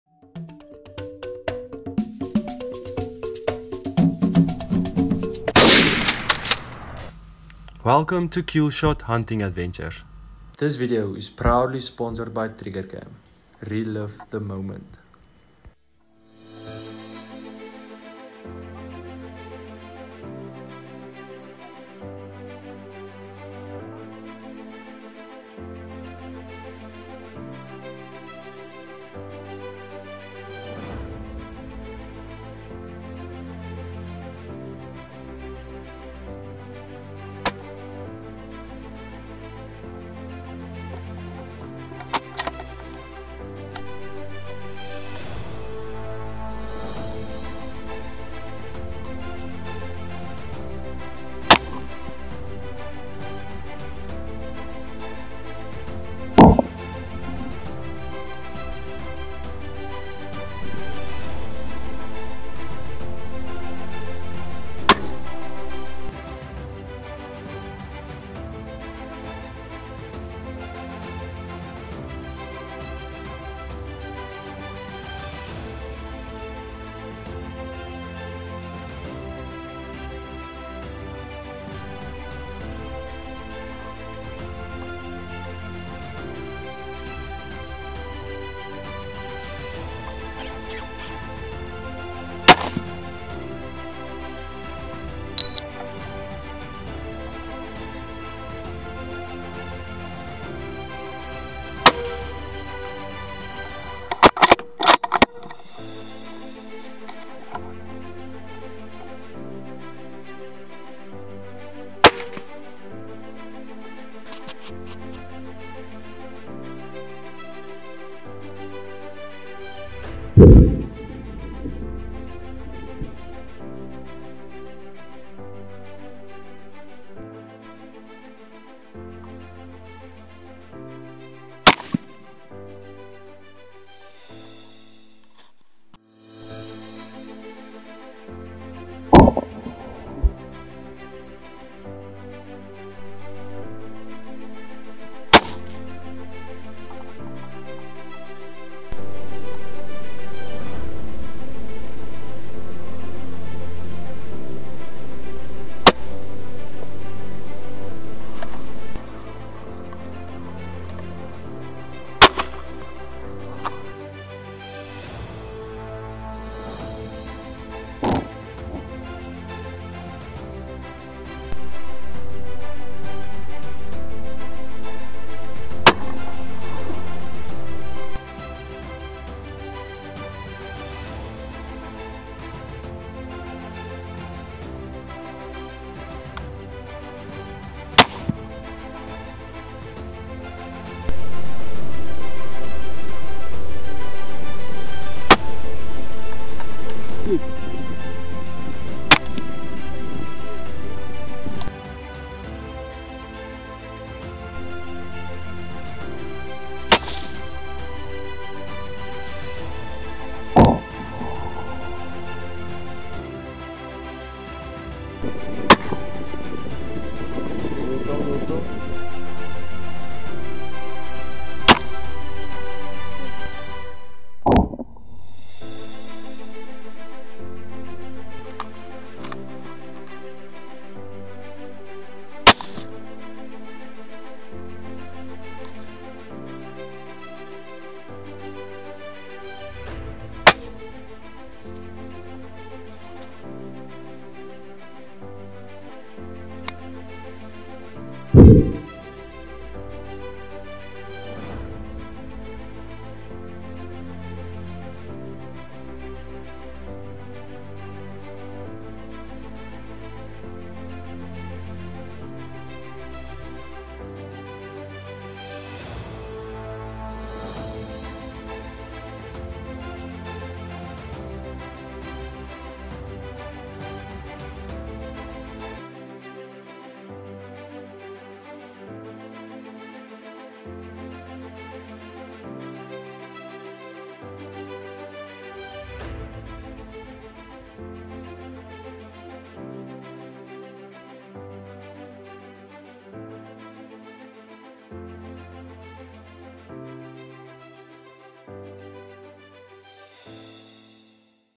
Triggercam Compilation #2 - Hunting Baboon , Warthog, Jackal, Springbuck
for more information.
Also Thanks for Nico Harris for some of the great video material. Please go and support his channel @ nico harris